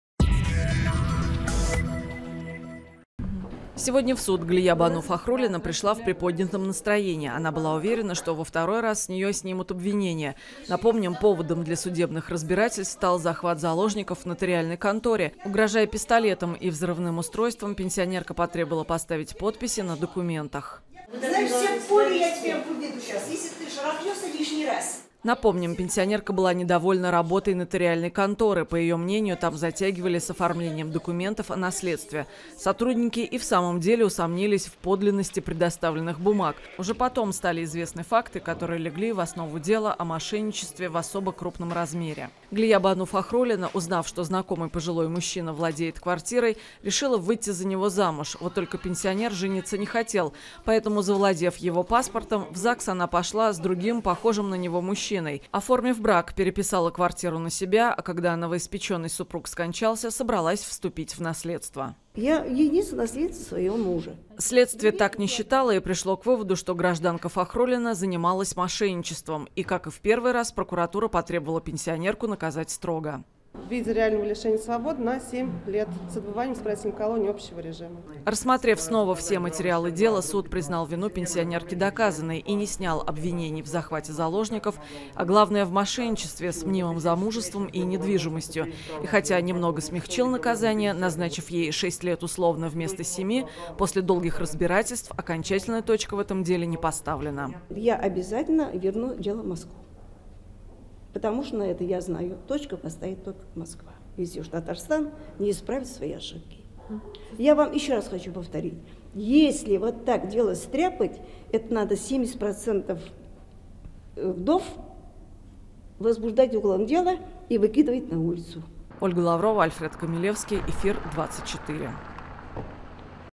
Верховный суд РТ вынес второй приговор пенсионерке, захватившей заложника
Она признана виновной в захвате и удержании лица в качестве заложника из корыстных побуждений, также в покушении на мошенничество и заведомо ложном доносе о совершении преступления.